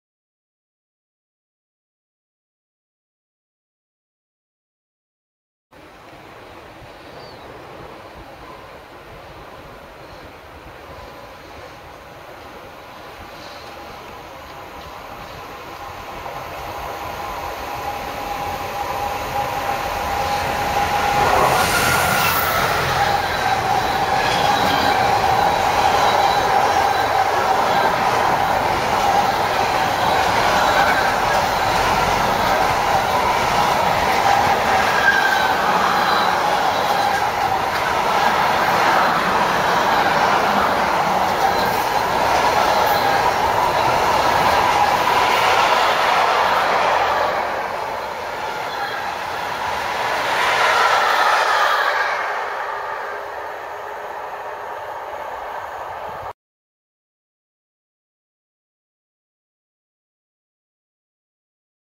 TXL 185 407-4 med trailertog
TXL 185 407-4 med trailertog passerer Kolding den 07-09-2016